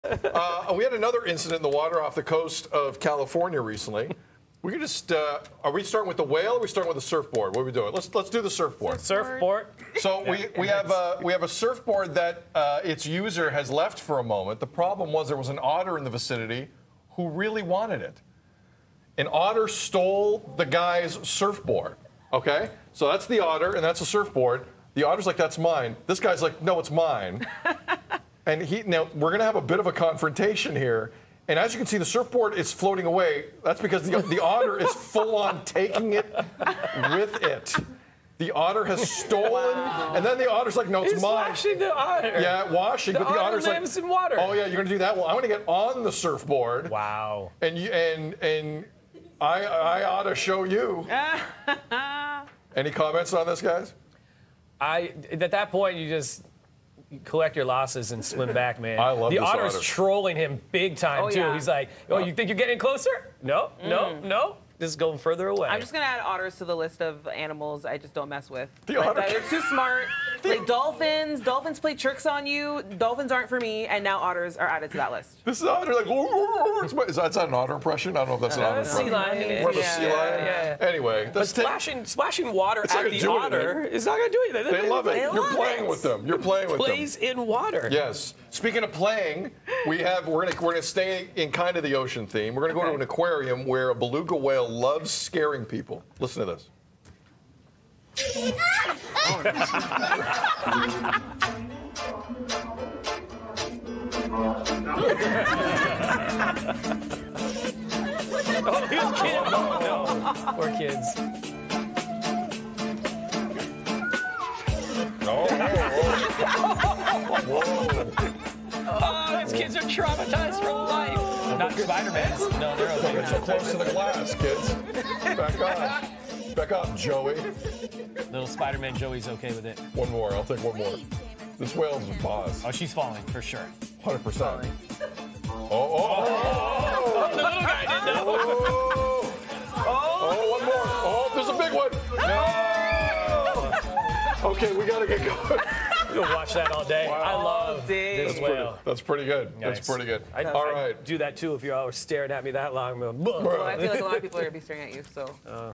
This beluga whale LOVES to scare aquarium visitors
Sid, Stephanie and Devo react to two viral water videos of an otter stealing a surfboard and a beluga whale that enjoys scaring people.